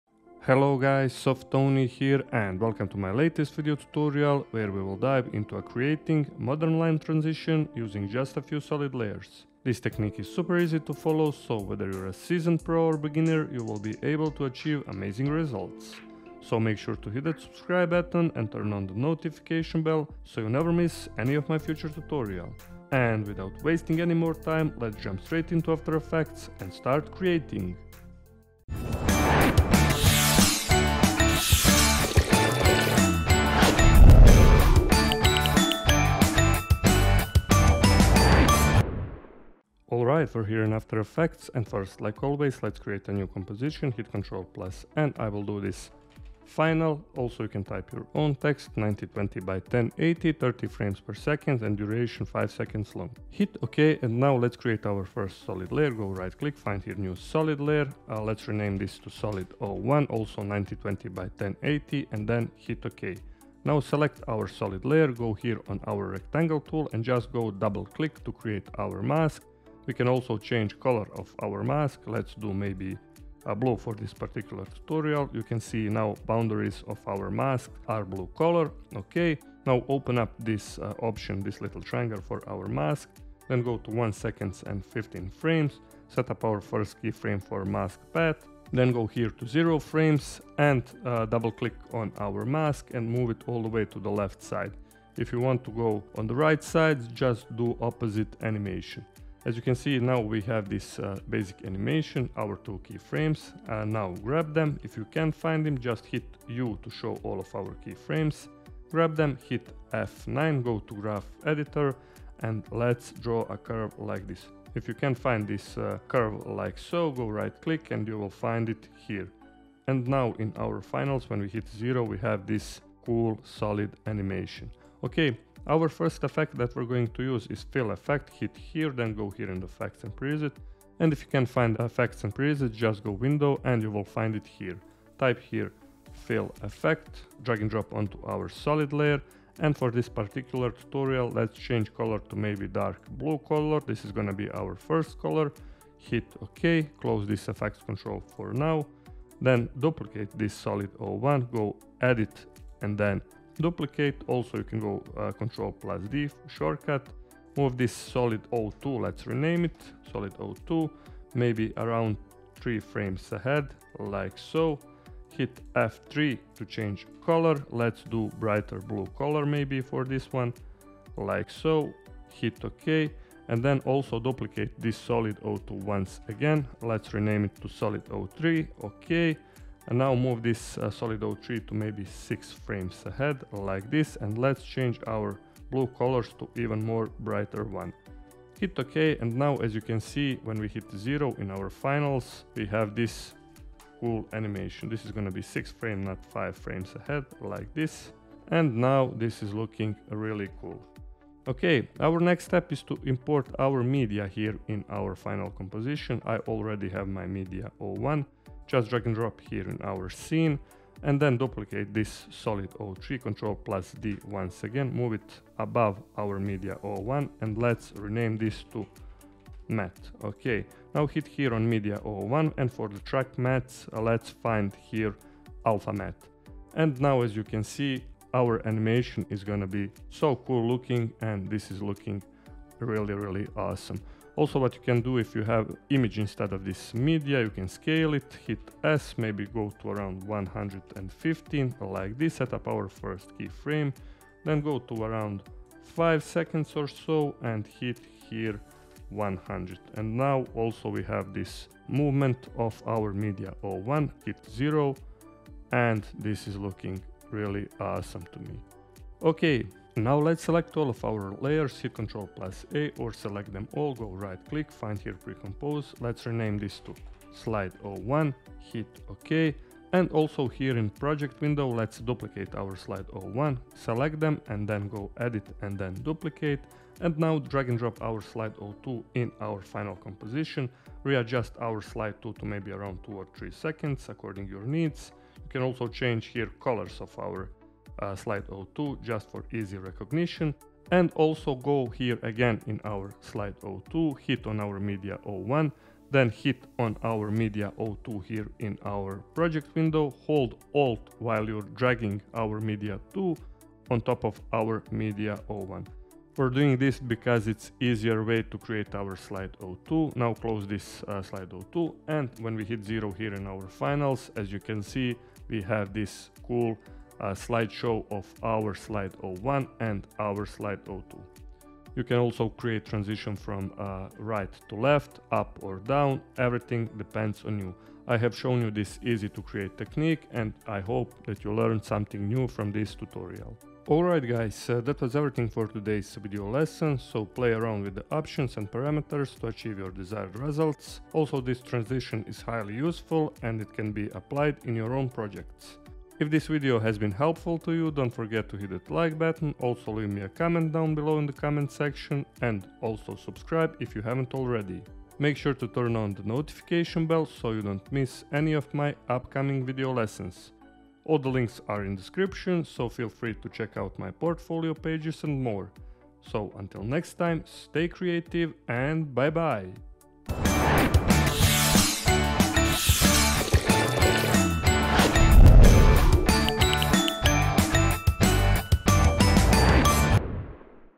Create LINE Transition in After Effects - After Effects Tutorial
Hey everyone, in this after effects tutorial I will show you how to create awesome and simple line transition for your own purpose - your slideshows, intros, promos, lower thirds in After Effects without using any plugins. This technique is a cool way to make your videos or images looking even better and to impress your audience. Very easy to make, just follow my steps!

*** My Portfolio - Stock Sites ***

*** Do you like music - Check out Senchi's portfolio ***